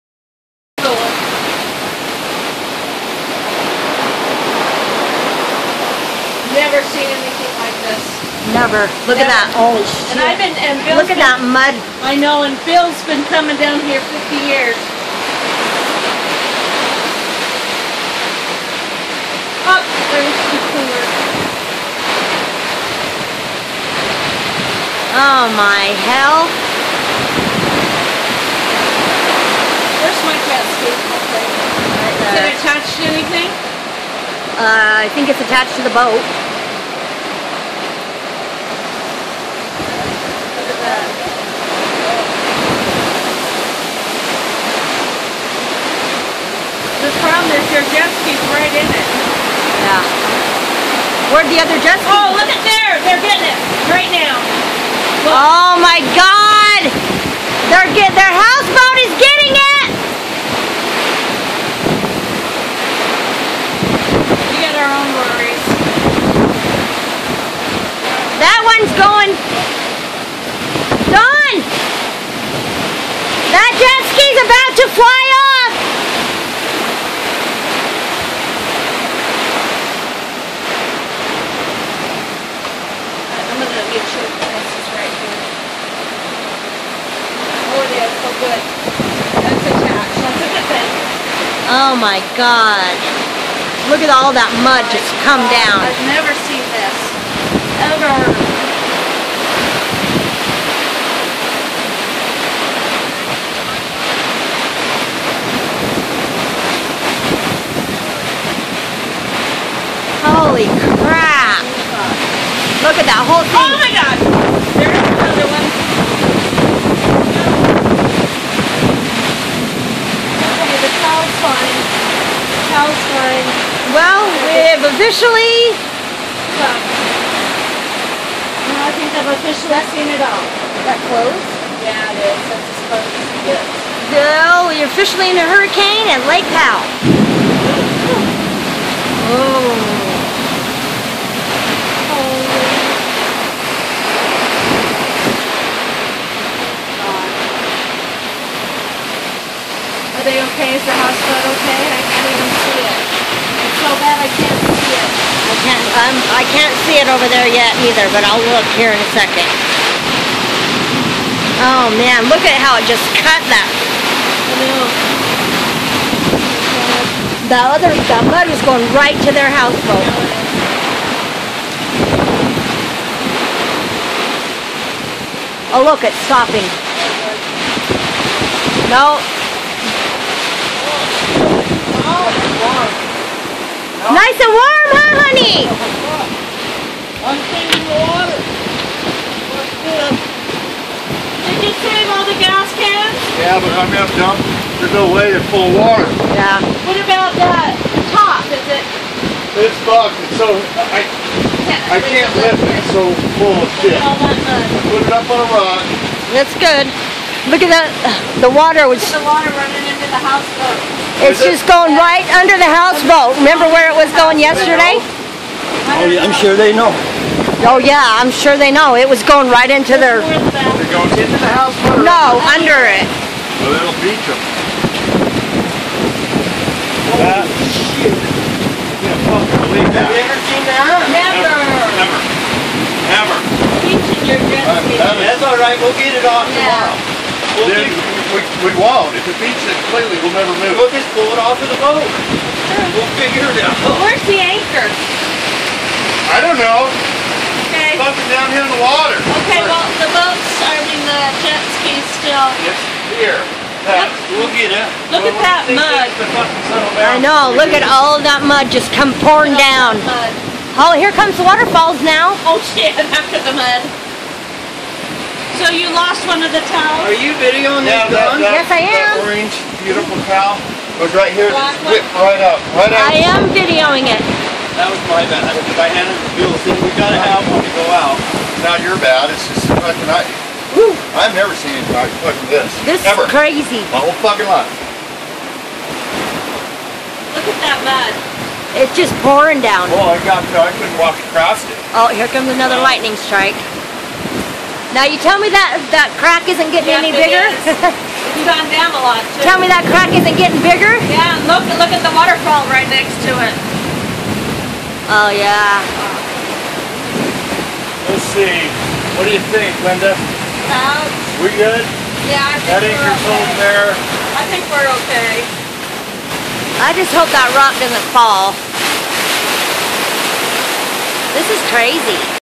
Lake Powell slick rock canyon flash flood p1.mp4
Thabks to Dawn M for shooting these videos.  this storm was at Lake Powell Slick Rock Canyon and shows the destruction of our beach. At several points we were truly in fear f so hence the bad language